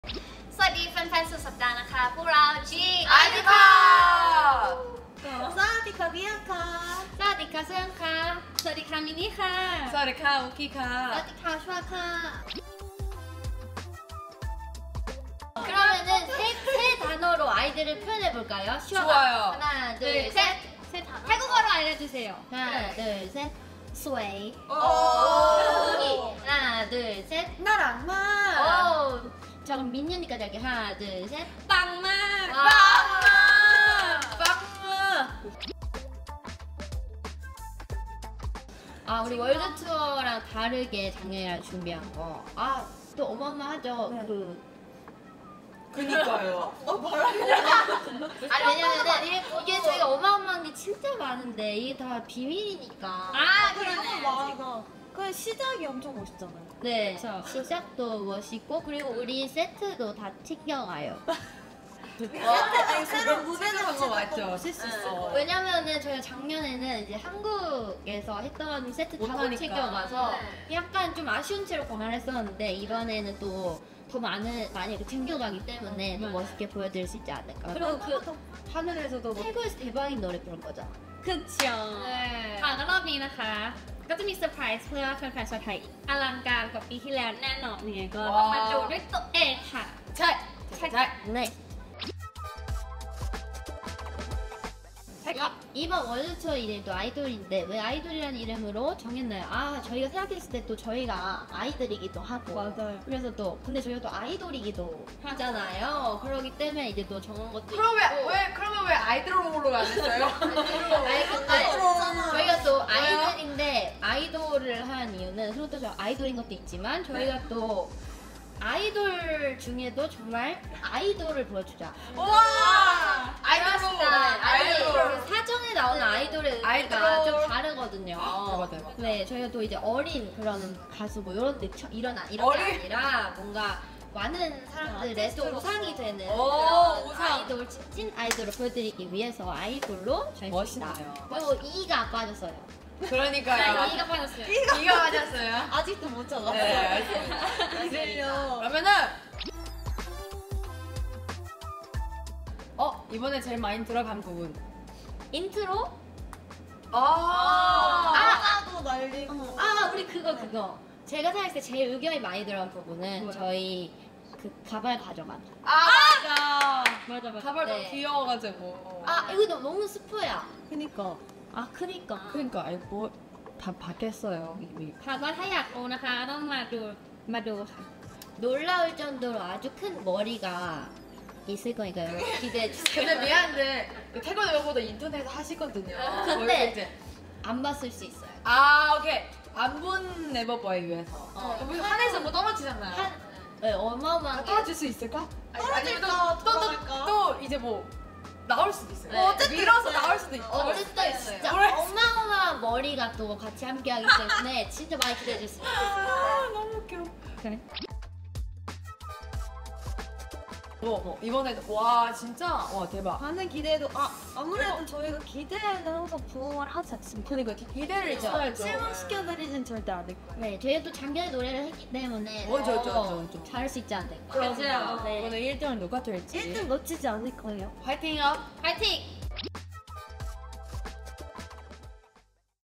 ชวนคุยกันแบบน่ารักๆ กับ 5 สาว (G)I-DLE
ชวนคุยกันแบบน่ารักๆ กับ 5 สาว (G)I-DLE ก่อนขึ้น คอนเสิร์ต “2024(G)I-DLE WORLD TOUR [iDOL]” ทุกคนเตรียมความพิเศษมาให้แฟนๆ ชาวไทยแน่นอน มาเจอ (G)I-DLE ได้ในวันเสาร์ที่ 19 ตุลาคม 2567ณ อิมแพค อารีน่า เมืองทองธานี ใครยังไม่มีบัตร จองบัตรได้ที่